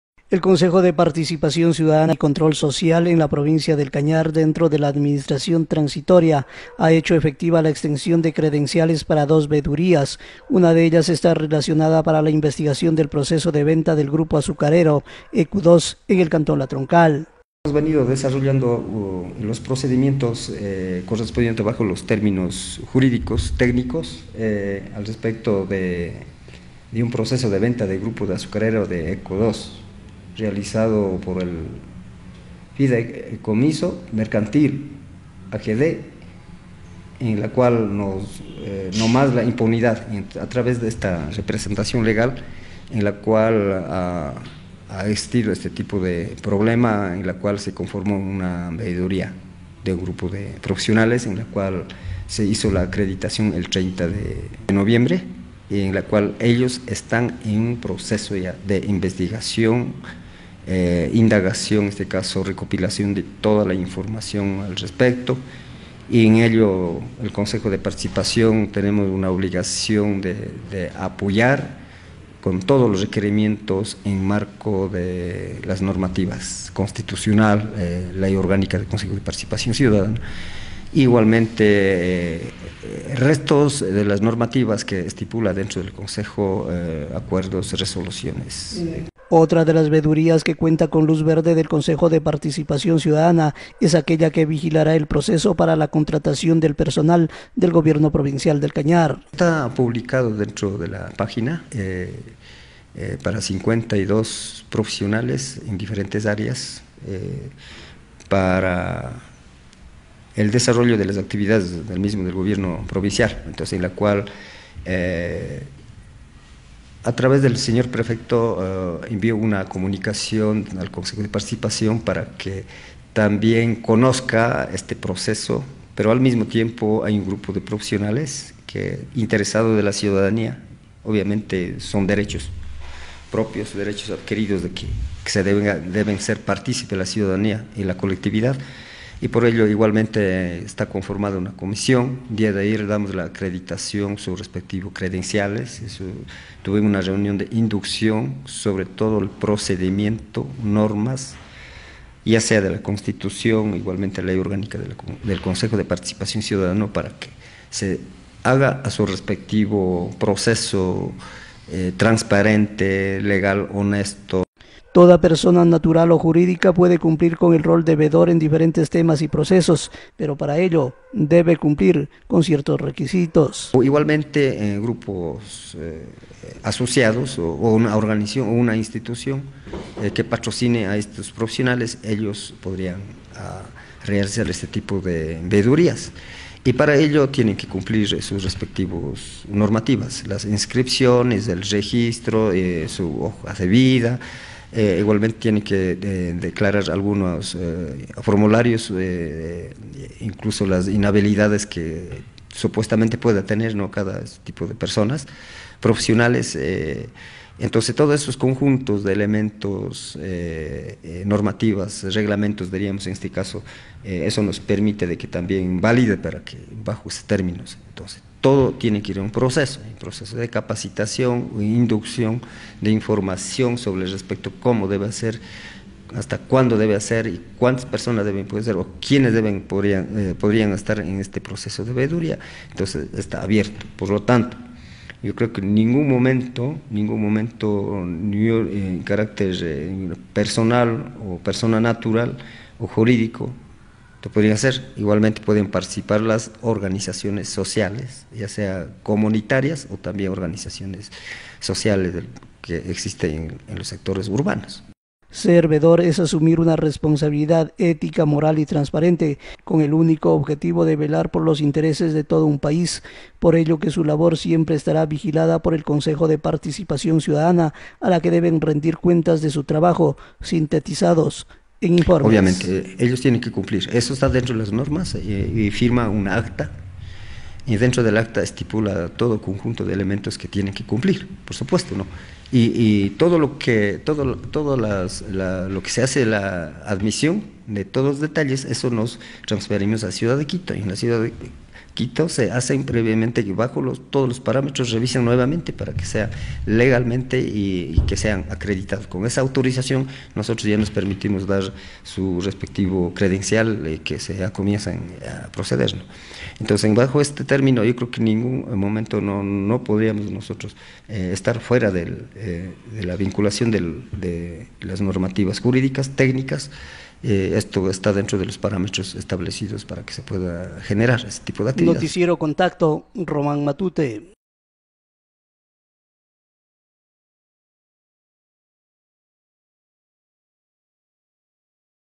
CPCCS realiza dos veedurias
A nivel de la provincia dos veedurías han sido acreditadas por parte del Consejo de Participación Ciudadana y Control Social, cumplirá su labor en la Troncal y la otra en la capital provincial.